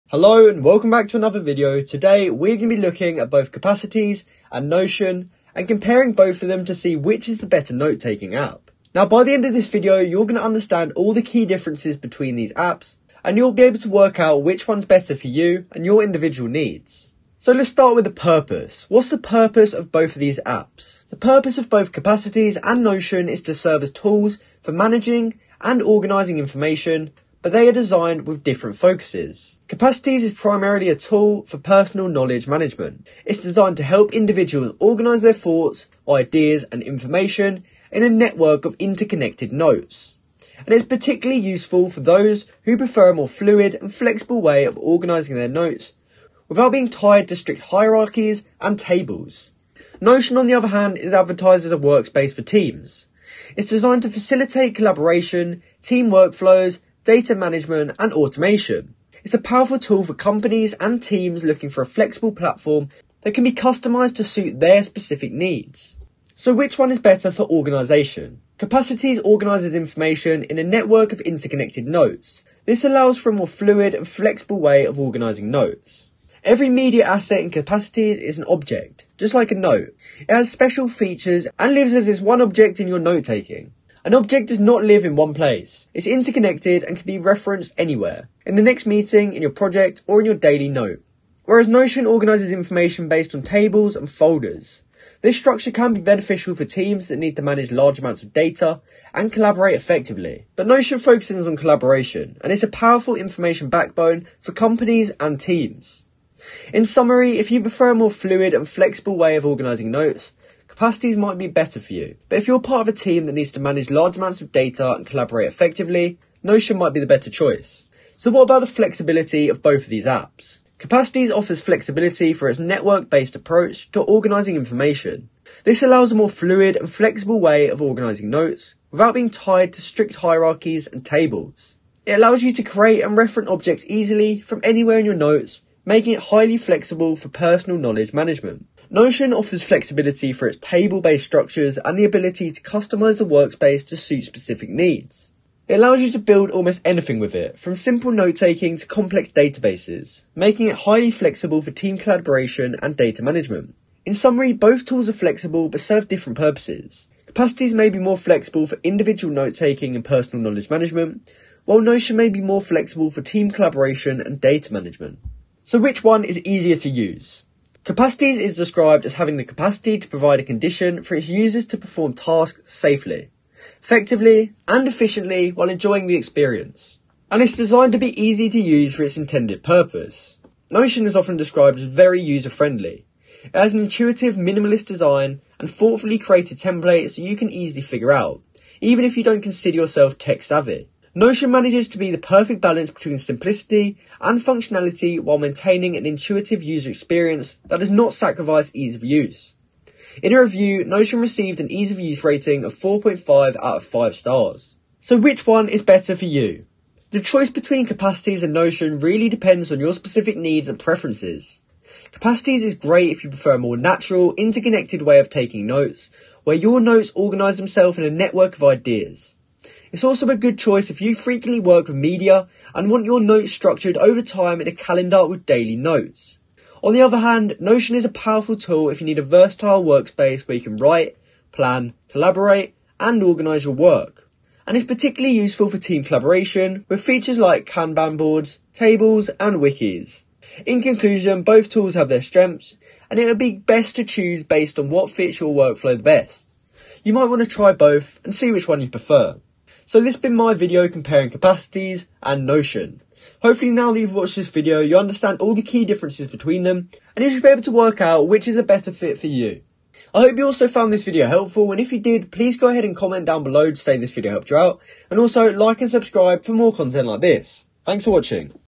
Capacities vs Notion: Which Note Taking App Is Better? (2025)
In today's video i will compare capacities and notion to see which is the better note taking app!